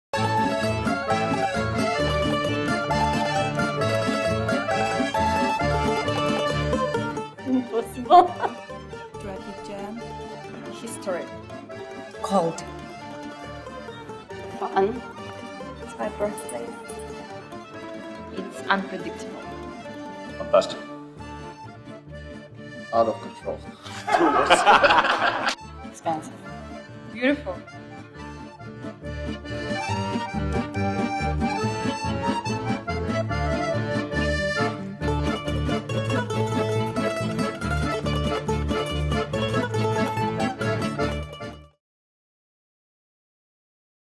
Russia's Style Elite Describes Moscow In One Word
In a short film by Emma Summerton, Moscow’s new style setters are subverting cliches to establish a bold new fashion order.